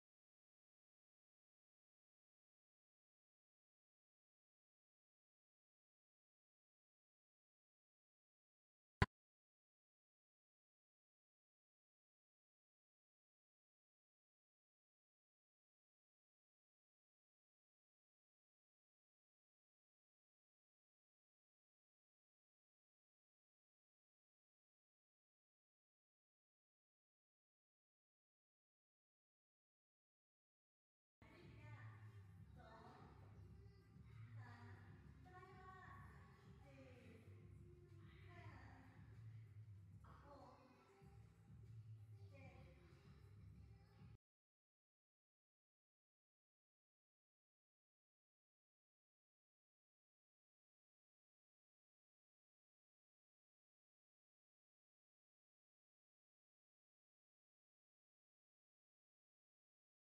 Body Weight@Bodyworkshatyai 02022018
บอดี้เวทเล่นง่ายทุกคนทำได้ ❗️แต่ถ้าเล่นคนเดียวมันก็จะเหนื่อยหน่อย
**เข้ากรุ๊ปคลาสกับเราซิค่ะ**
 ครูแมนสอนดี เพื่อนน่ารัก สนุกสนานกันทั้งชั่วโมง